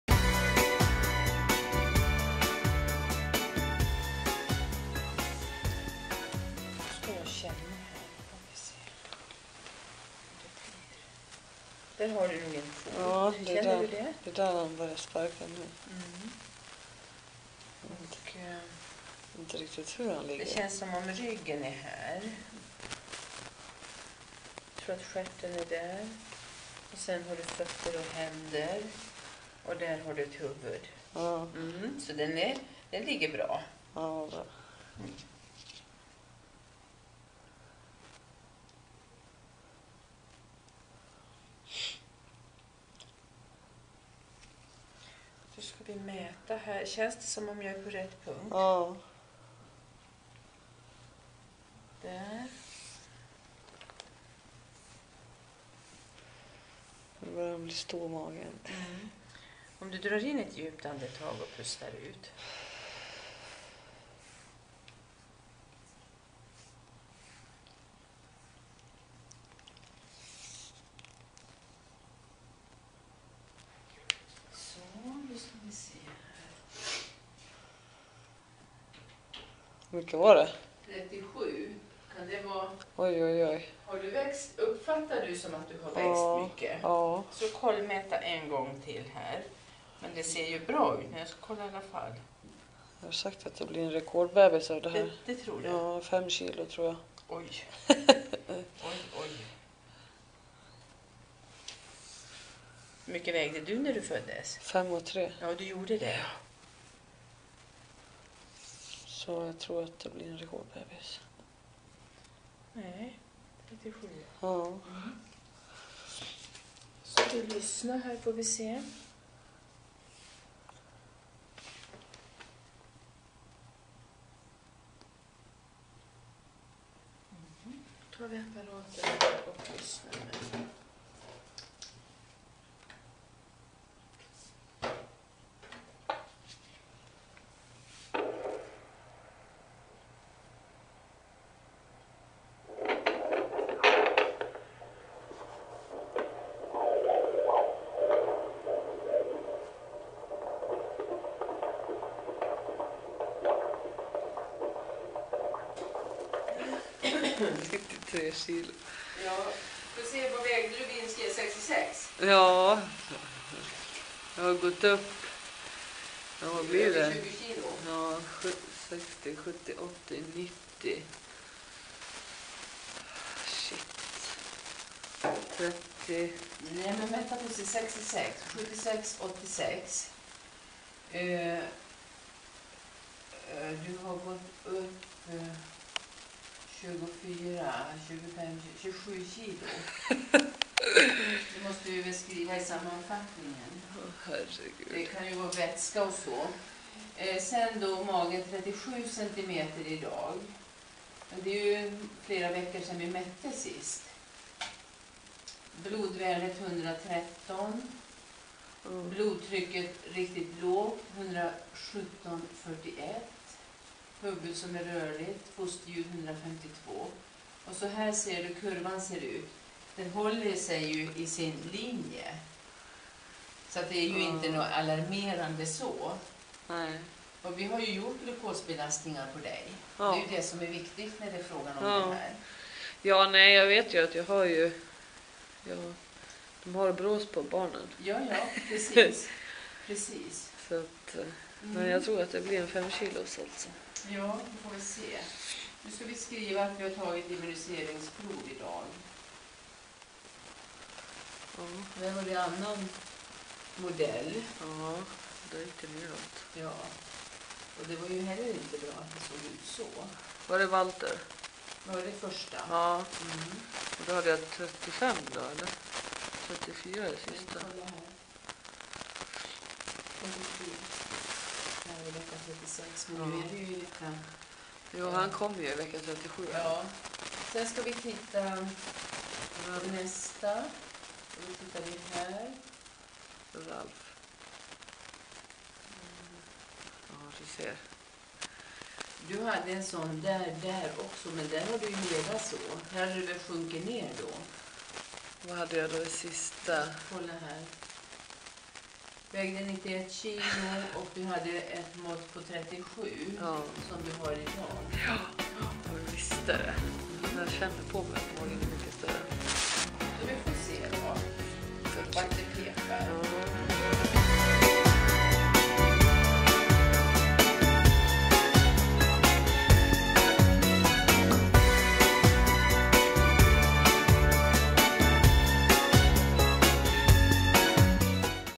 Barnmorskebesök - Nikolina v.37
Nikolina är på barnmorskebesök och snart är det nära! Se hur din bebis utvecklas med Libero-appen, ladda ner den här:

I samarbete med Libero.